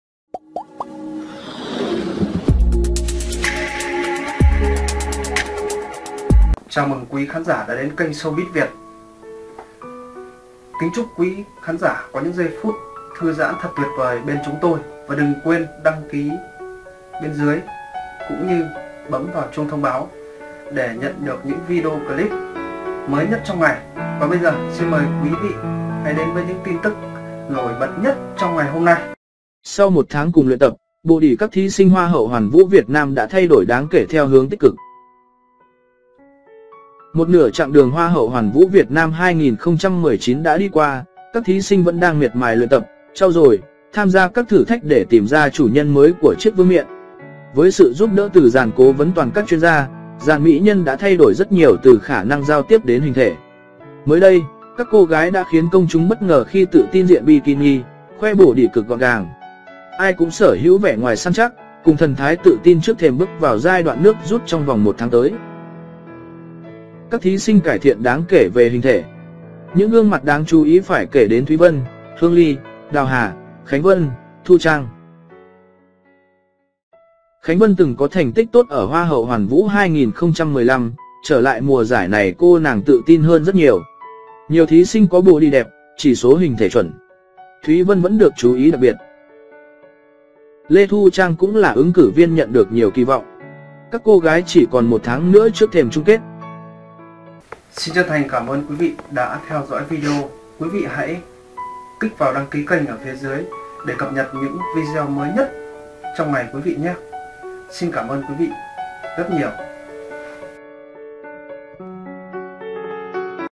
BẤT NGỜ với body lột xác đáng kể trong bikini của dàn thí sinh mạnh nhất Hoa hậu Hoàn vũ Việt Nam
Sau 1 tháng cùng luyện tập, body các thí sinh Hoa hậu Hoàn vũ Việt Nam đã thay đổi đáng kể theo hướng tích cực.